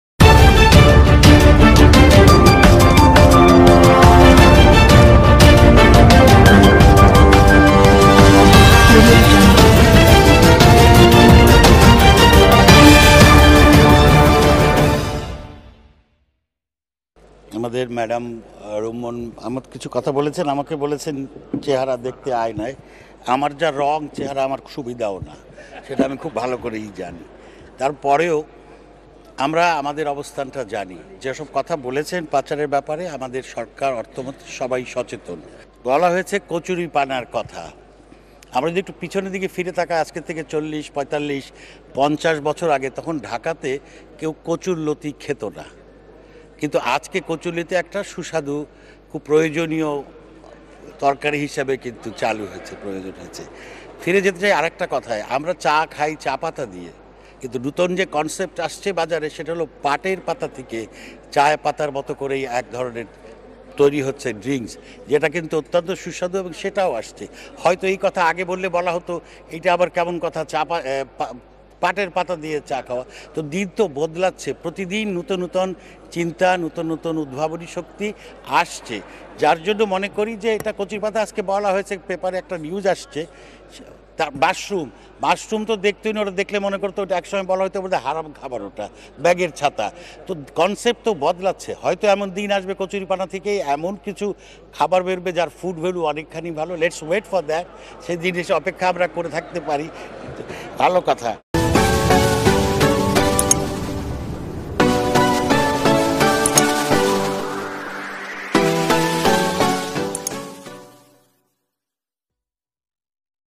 কচুরিপানার মধ্যে কি কি ভিটামিন আবিষ্কার হচ্ছে জেনে নিন পরিকল্পনামন্ত্রীর কাছ থেকে।LALTARANEWSBD
কচুরিপানার মধ্যে কি কি ভিটামিন আবিষ্কার হচ্ছে জেনে নিন পরিকল্পনামন্ত্রীর কাছ থেকে।